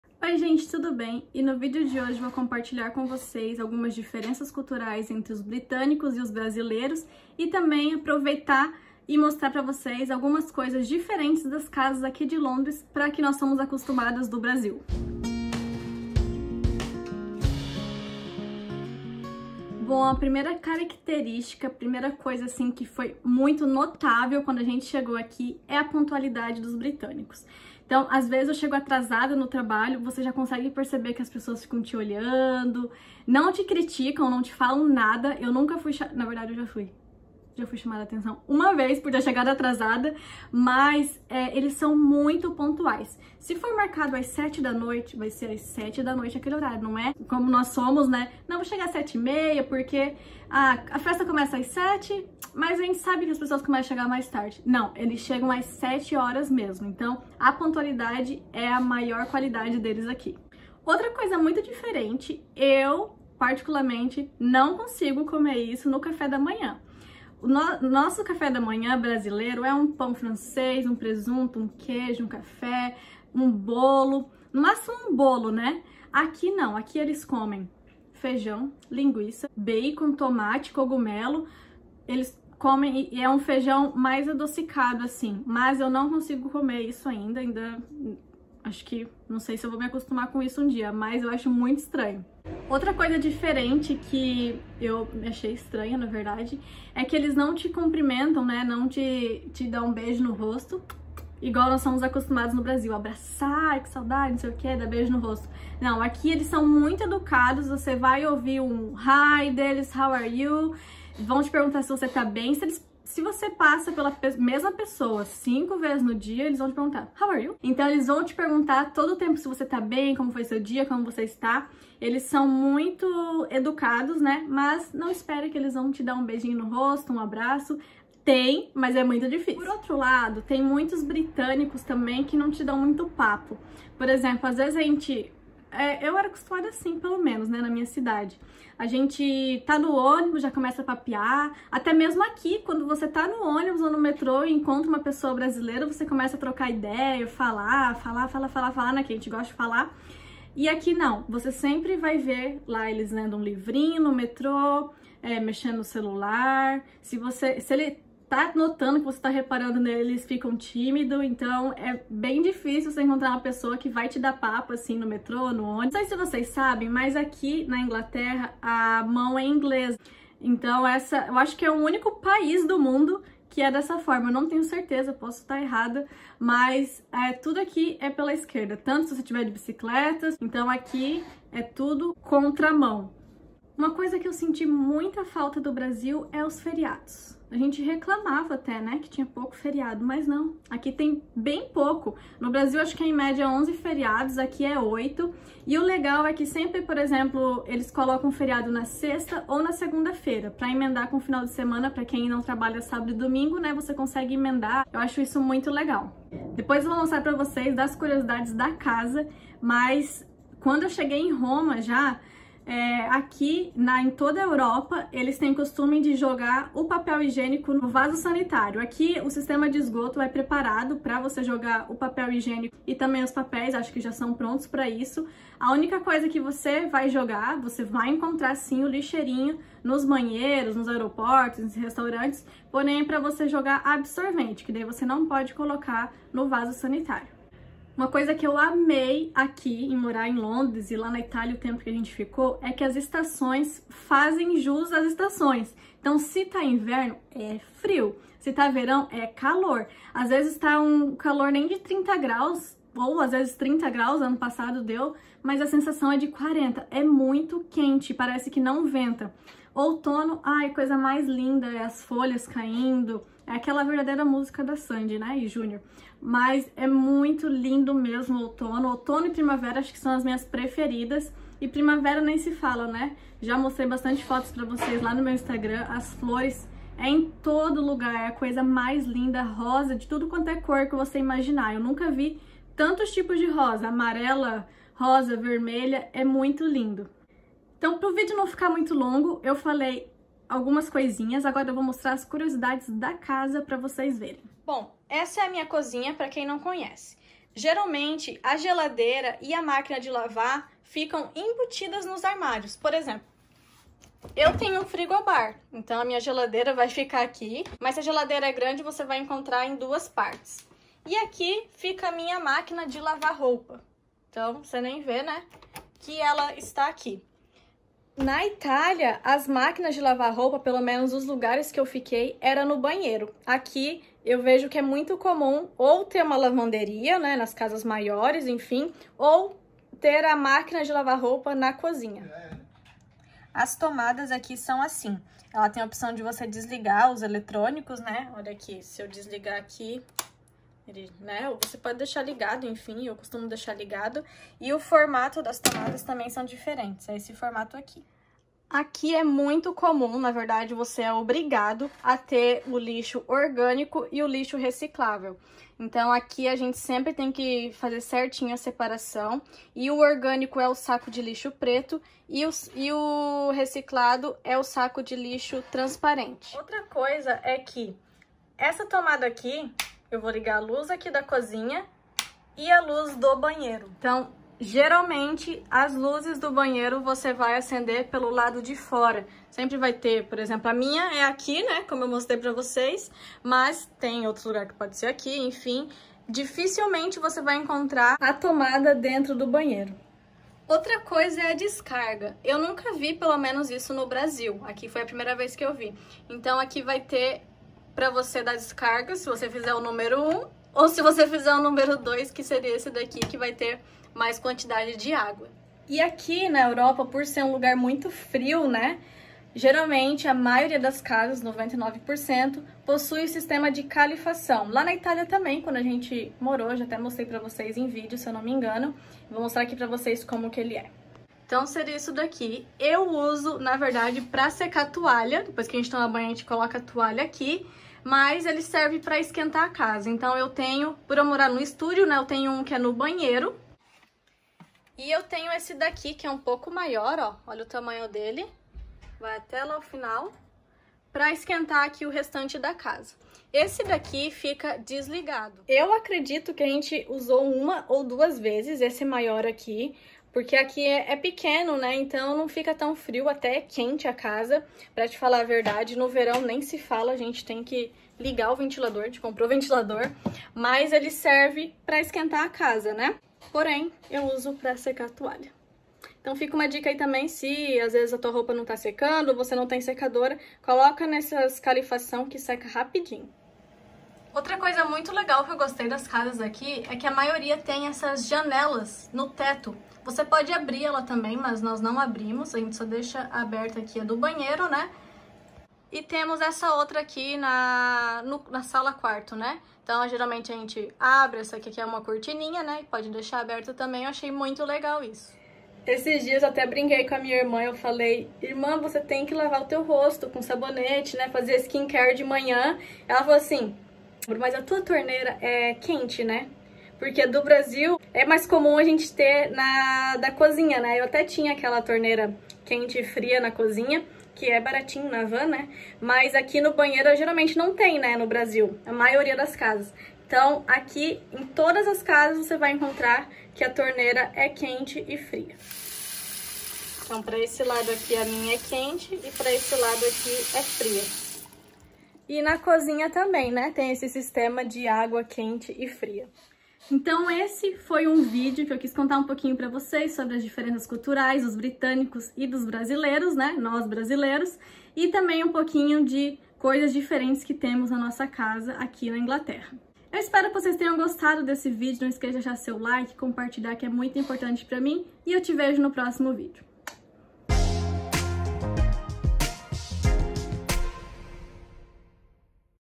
🇬🇧 Cultura britânica & um pouco das casas na Inglaterra
Oii gente, nesse vídeo contei pra vocês as culturas dos britânicos e mostrei também algumas coisas diferentes das casas da inglaterra.